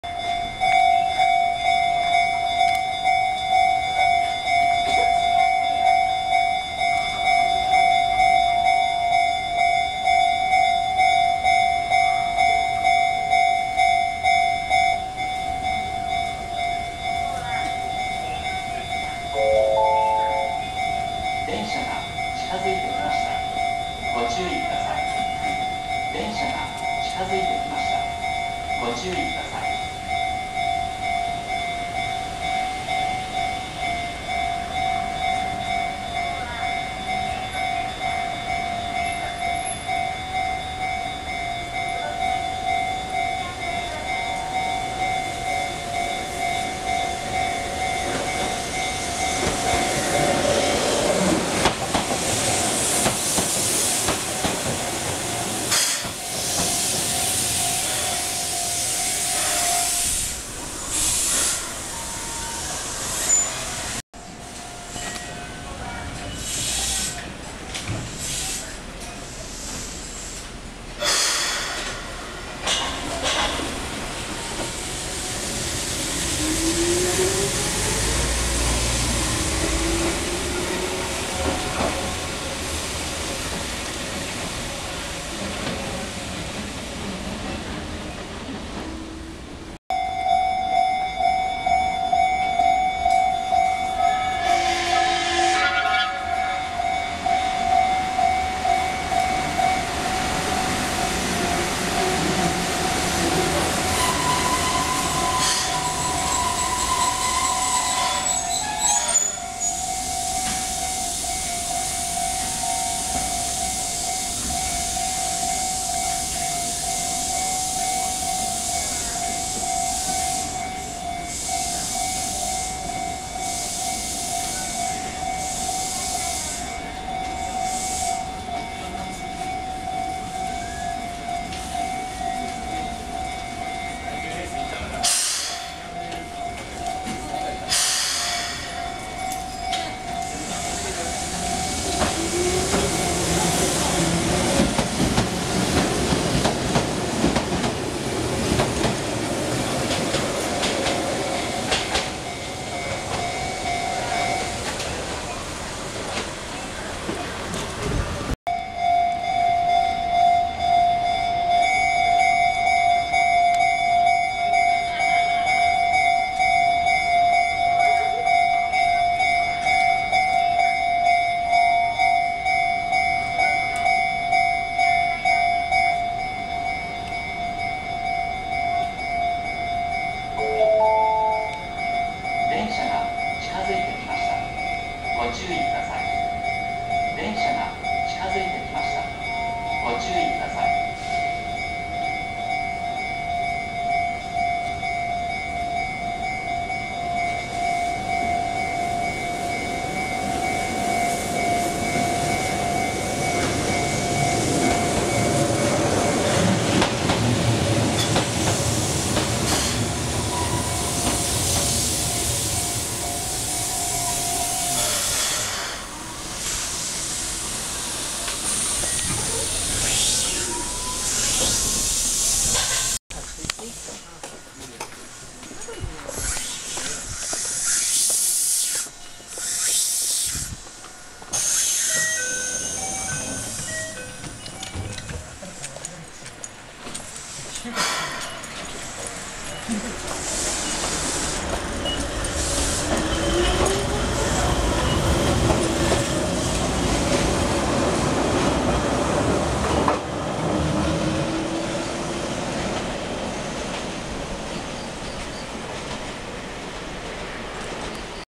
京阪 石山坂本線 600形 603-604 京阪旧塗装 609-610 内装更新車 京阪膳所 滋賀里 20191030
京阪 石山坂本線 600形
603-604 京阪旧塗装
609-610  内装更新車
京阪膳所  滋賀里